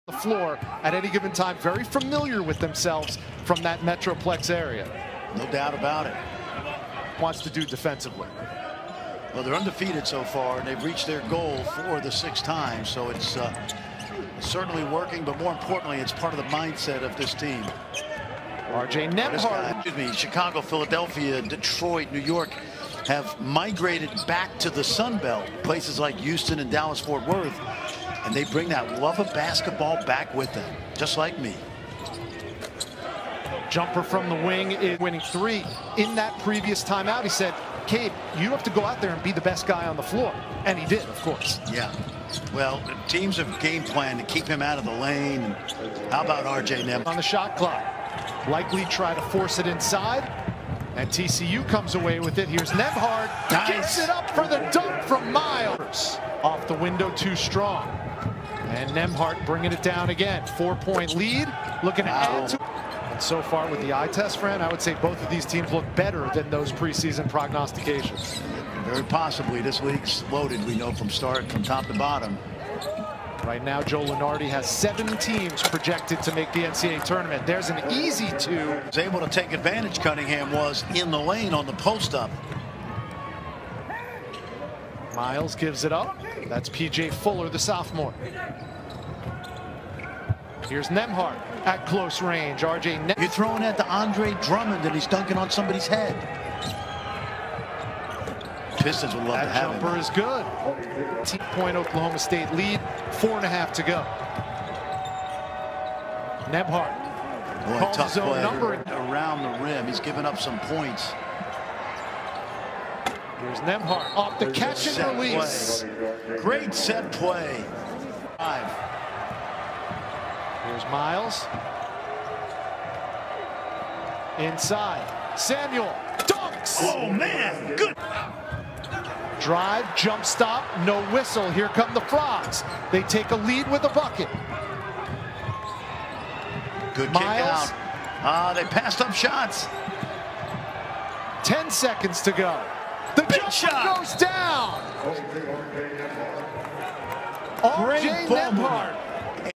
CJ Nembhard was CLUTCH vs OSU! 21 PTS 5 AST 5 REB
As a OU fan watching this game was extremely satisfying, seeing CJ outplay Cade was a sight to see and I will remember for the rest of Cade’s career, congrats to try for the win!